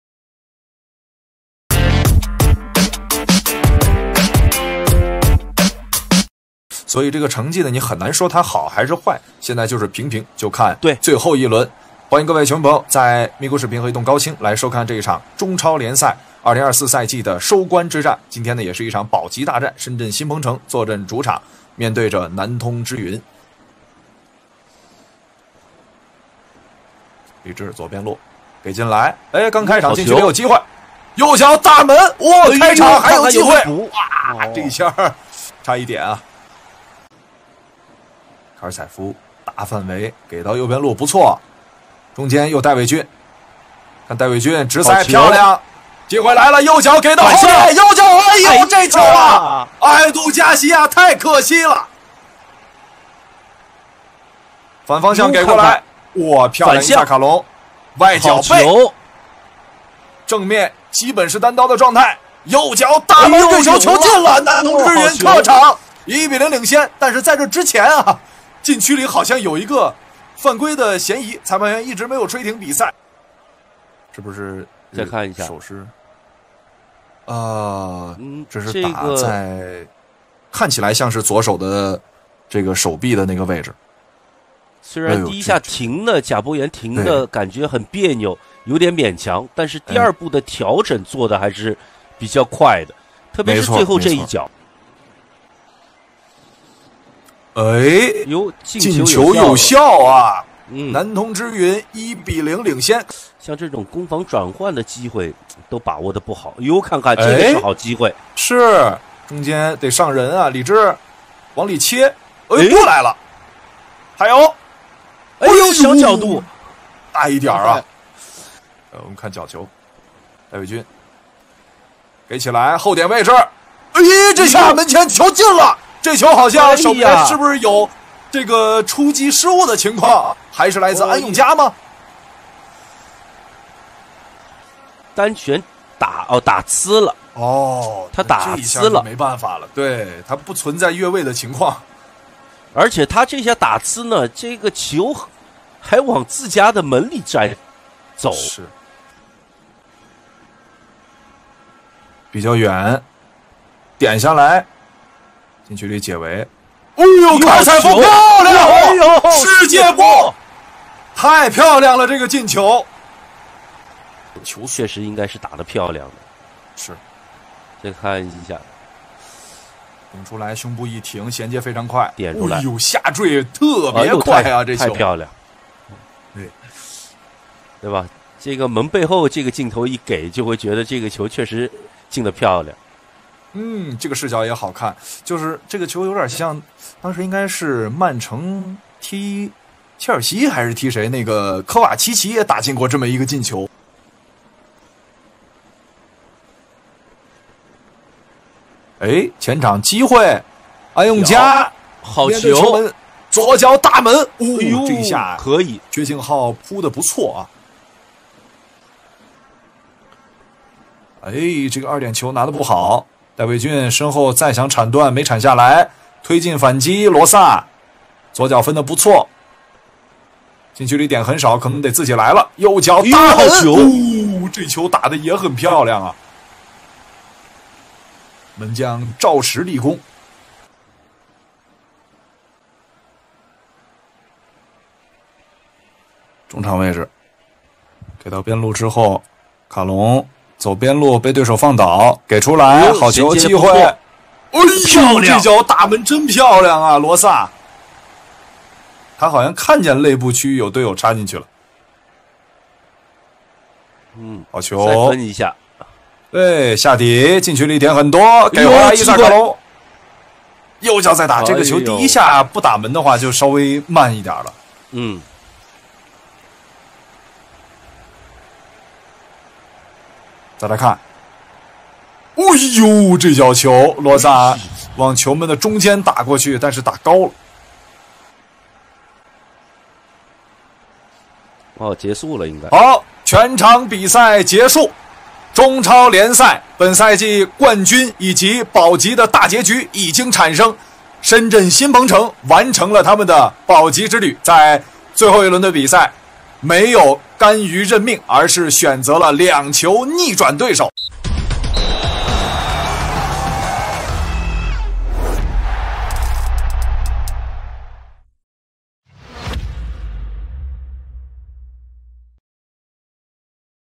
全场集锦 深圳新鹏城vs南通支云 2024中超联赛第30轮 HIGHLIGHTS Shenzhen Peng City vs Nantong Zhiyun CSL 2024 RD30
全场集锦 深圳新鹏城vs南通支云 2024中超联赛第30轮

HIGHLIGHTS Shenzhen Peng City vs Nantong Zhiyun Chinese Super League 2024 RD30